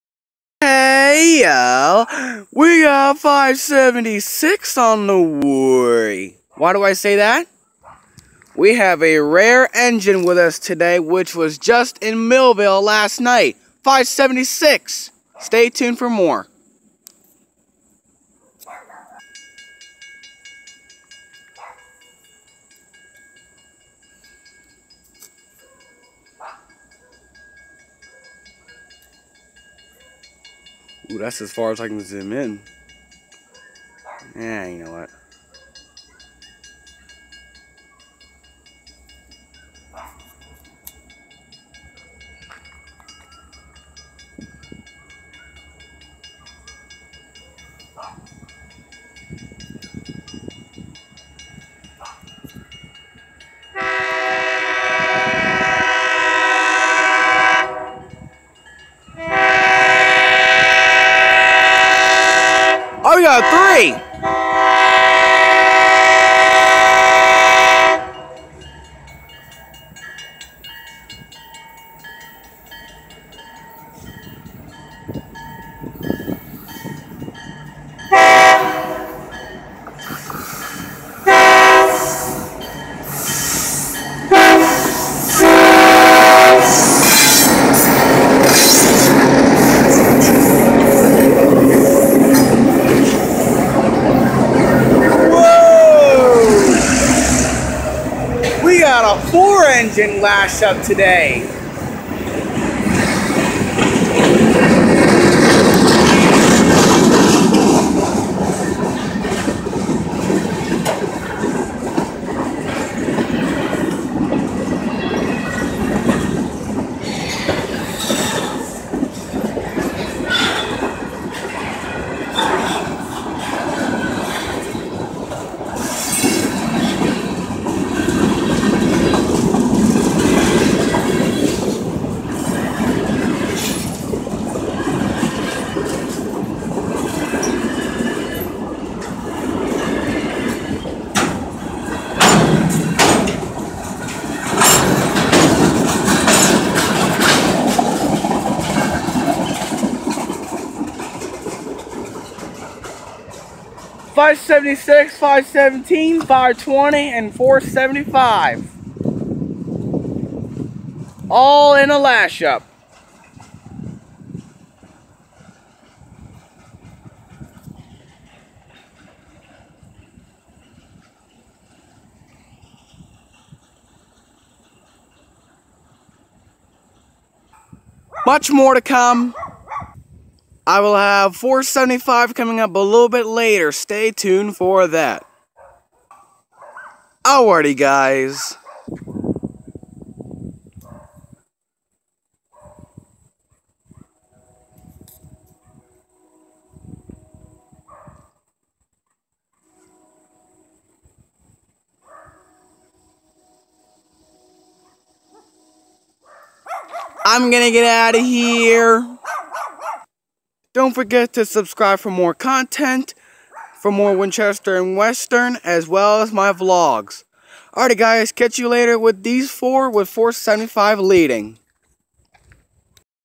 Winchester & Western in Fairton 576/517/520/475
Rare! 520/475 are joined up with 576/517!! Yesterday's trains August 6th. Also I am recording with a new phone, an LG G Stylo. Enjoy!